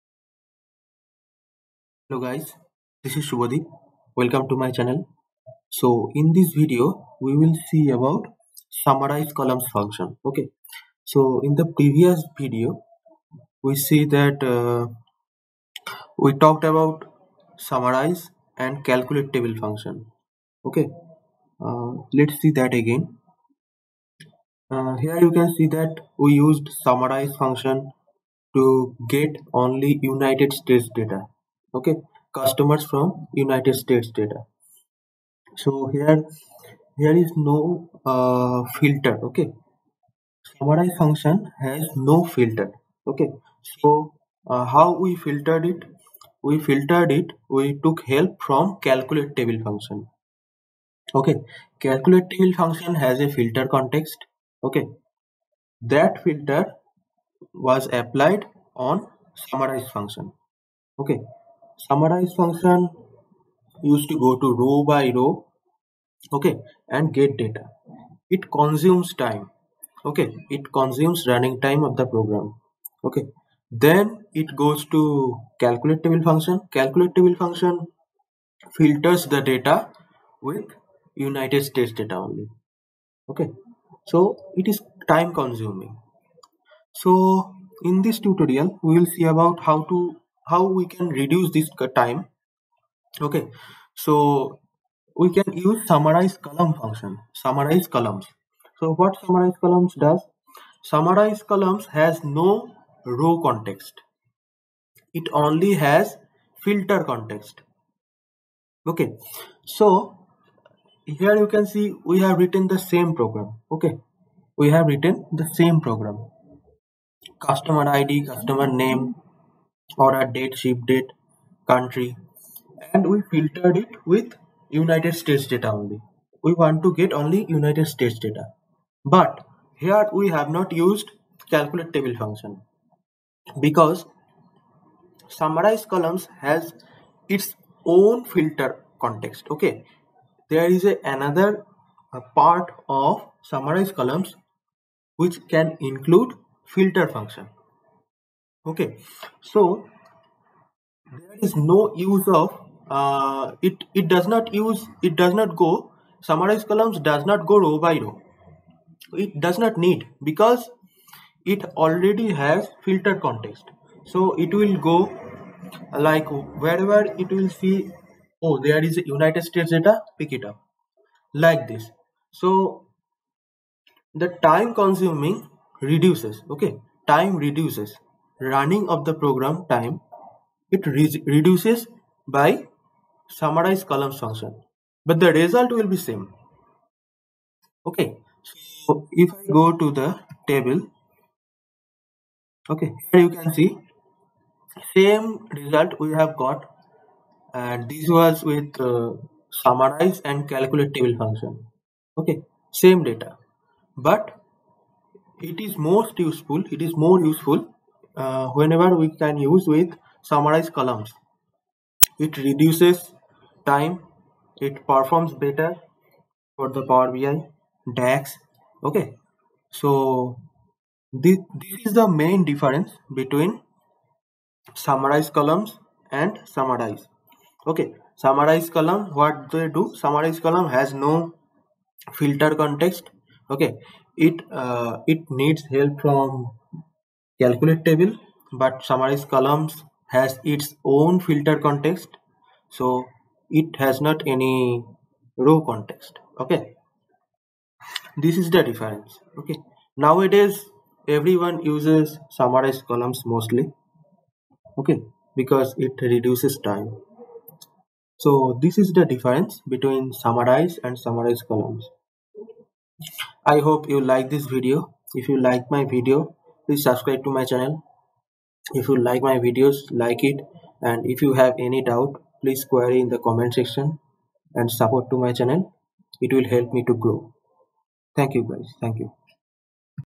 8) Use of summarizecolumns function instead of calculatetable and summarize
This video is about to showcase difference between summarize and summarizecolumns function.